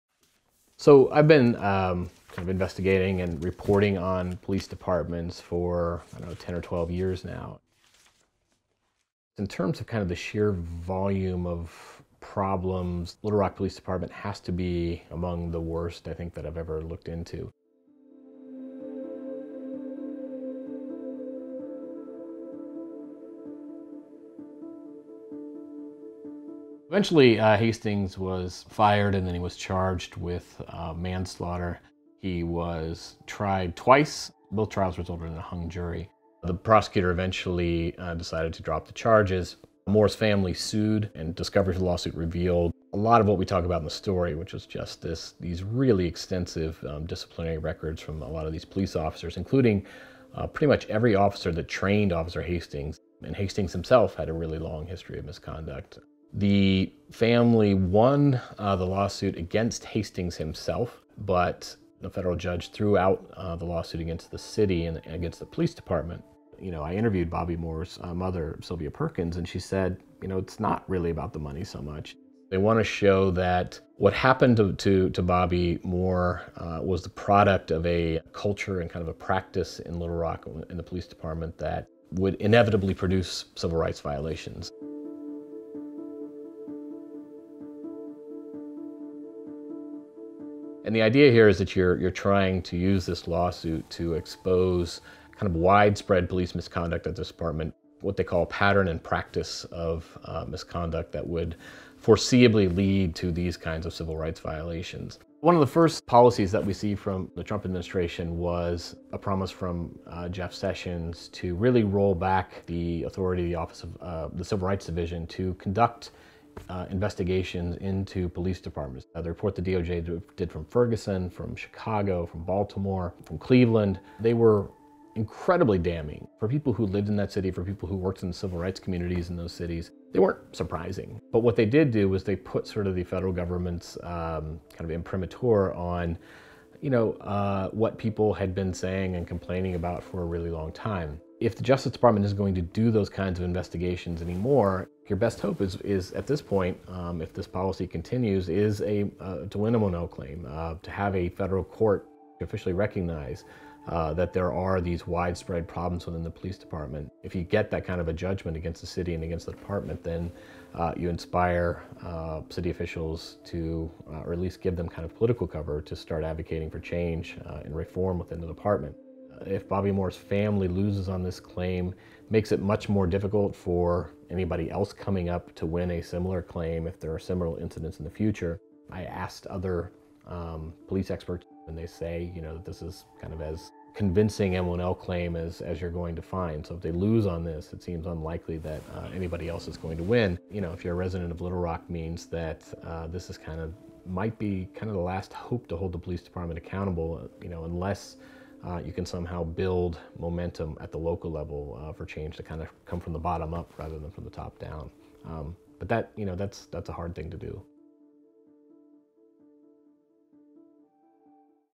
Opinion | Exposing police misconduct in Little Rock isn't hard. Finding justice is.
Radley Balko has been investigating U.S. police departments for more than a decade. He says tracking the career of one particularly problematic officer in Little Rock revealed a horror show of misconduct, cover-up and cascading institutional failure.